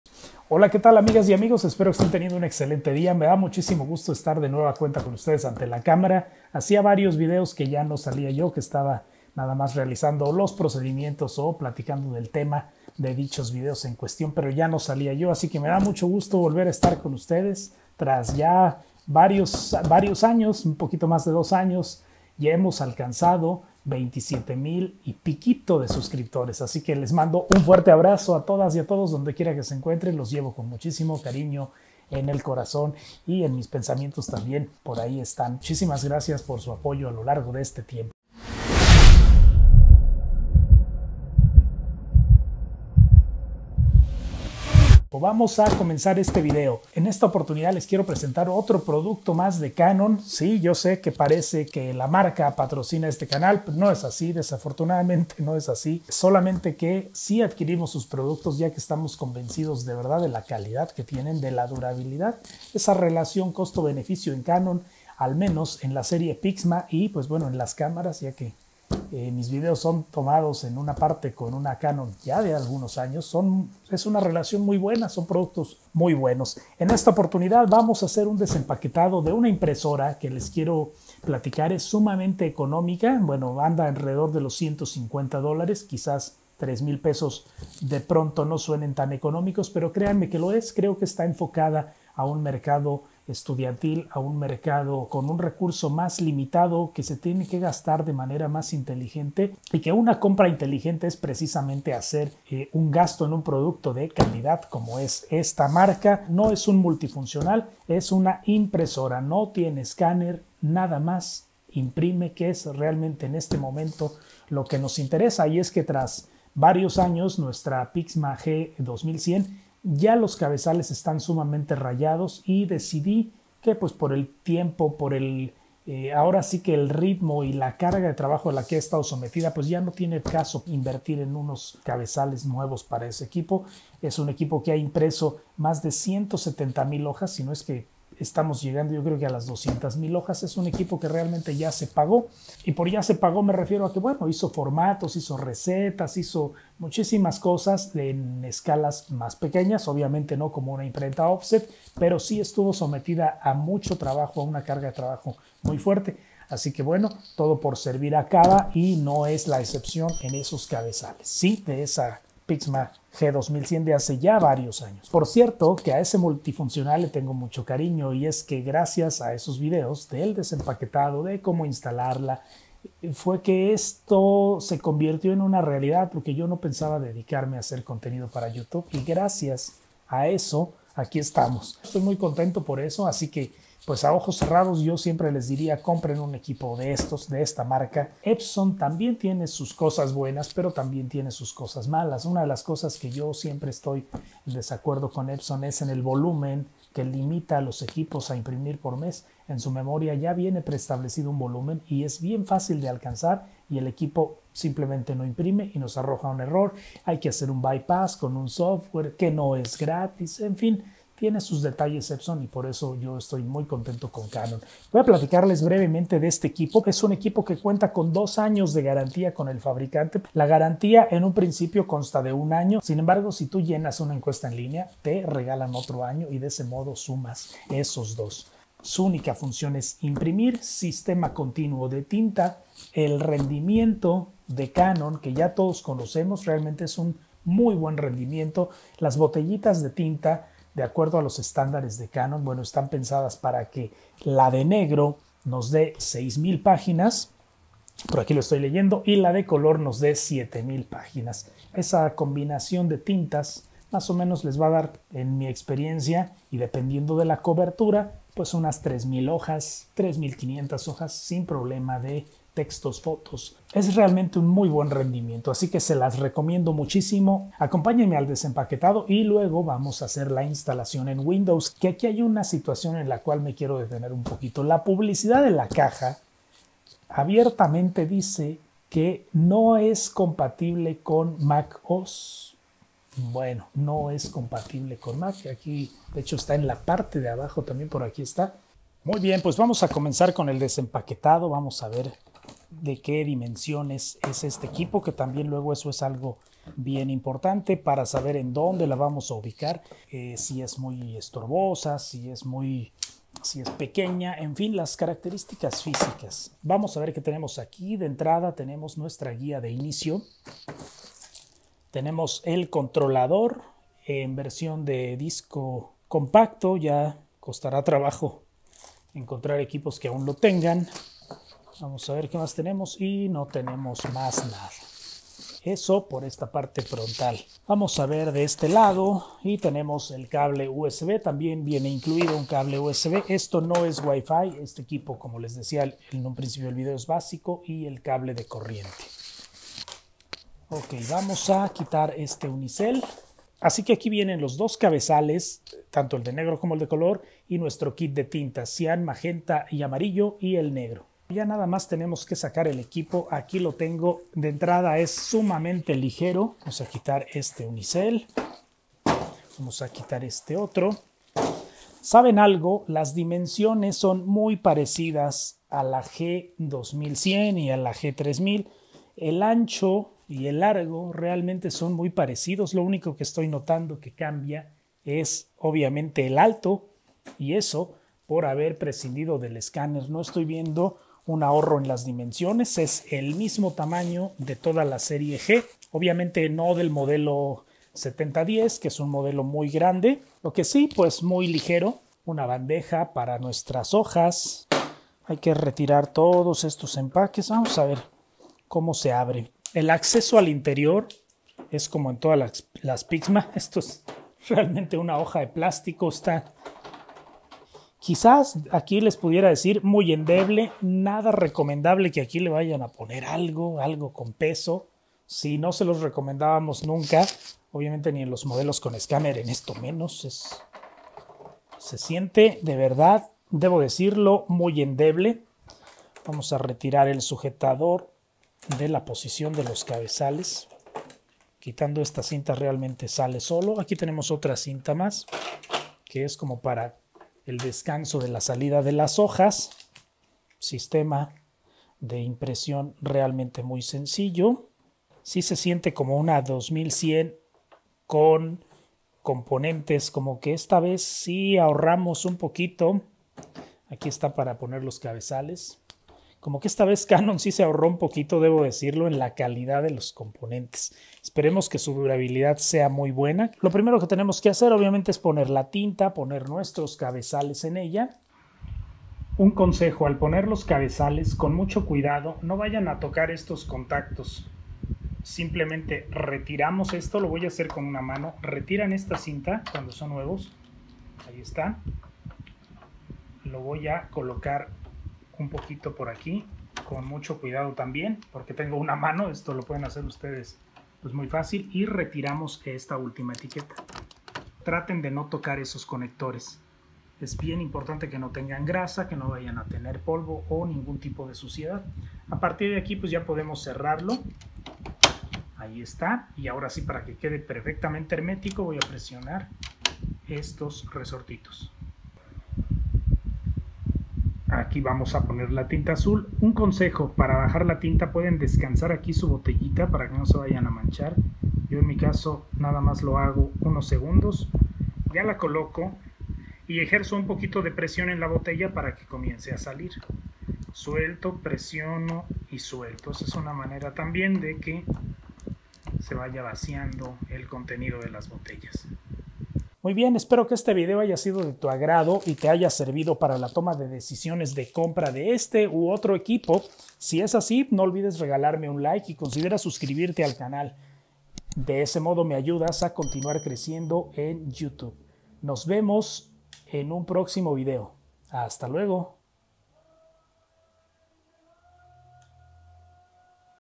👌 Canon Pixma G1110, 🖨 impresora económica y de calidad 👨🏻‍💻
Compramos esta impresora Canon Pixma G1110, este es el desempaquetado y opinión sobre este producto ideal para estudiantes o si no ocupas el escáner pero quieres la calidad y durabilidad de esta marca.